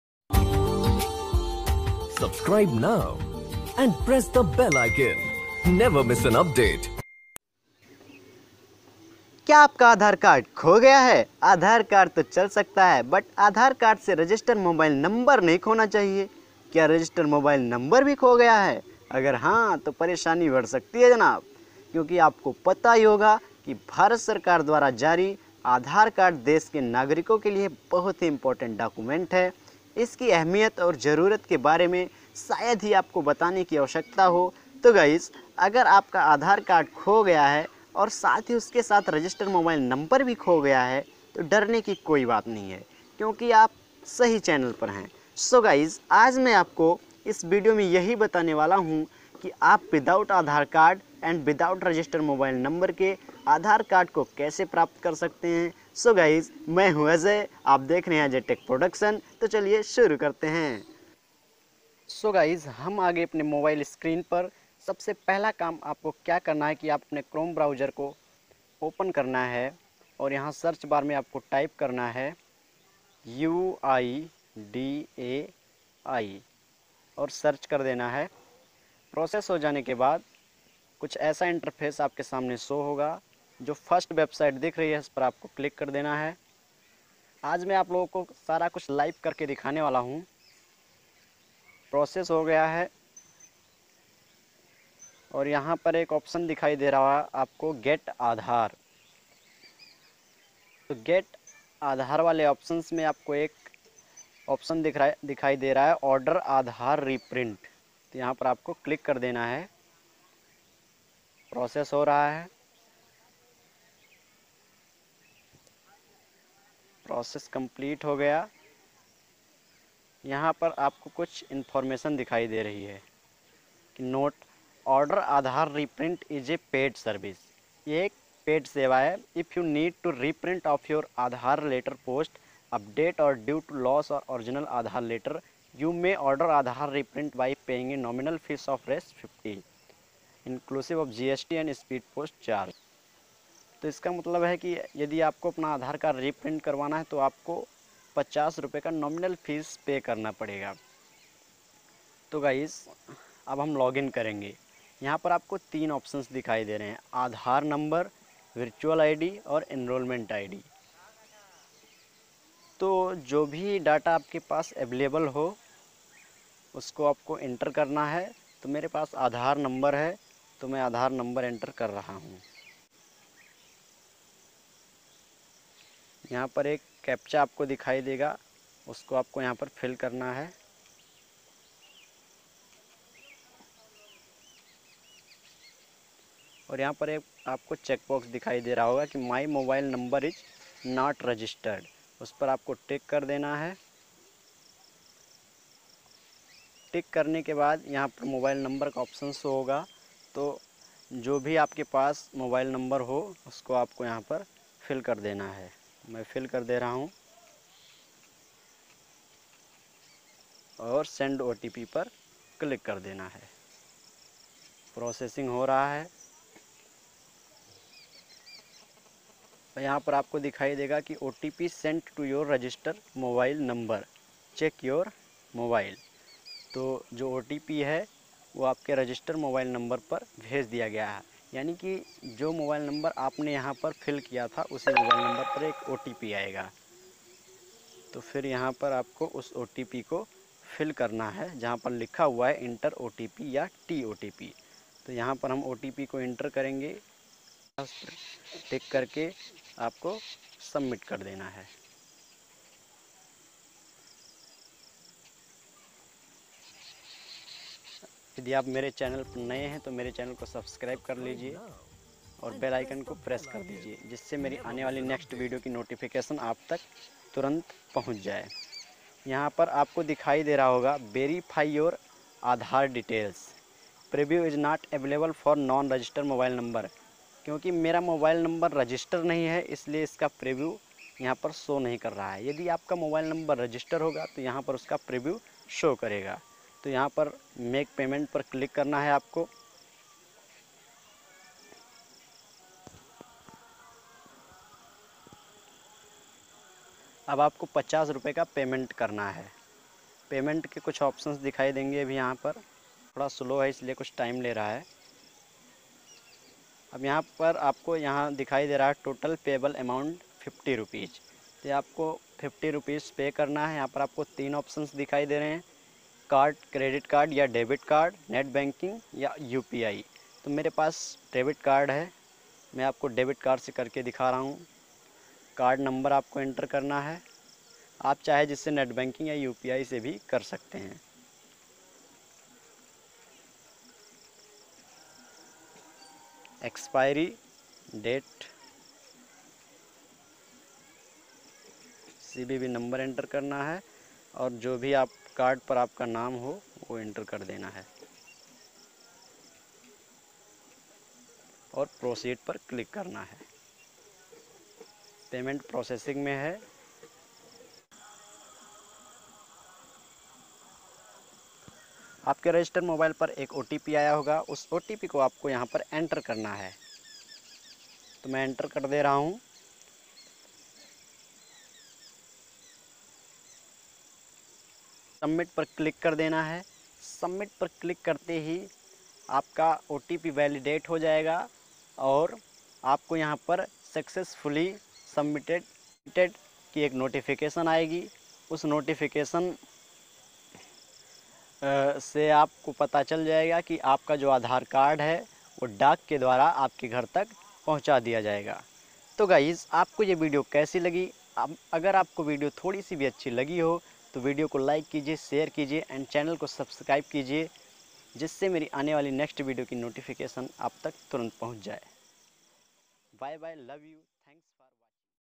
Aadhaar card Download without Mobile Number & Aadhaar card 2020||Aadhaar card Download kaise kare?
Hey Guys,Welcome to AJAY TECH PRODUCTION. So Guys, Aaj main aapko without Aadhaar Card and without Mobile Number ke Aadhaar Card Download karne ka tarika batane wala hu woh bhi live.

Aadhaar card download without Mobile Number and Without Aadhaar card in 2020

Aadhaar card download kaise kare?

++--AJAY TECH PRODUCTION--++

+---+---THANKS---+---+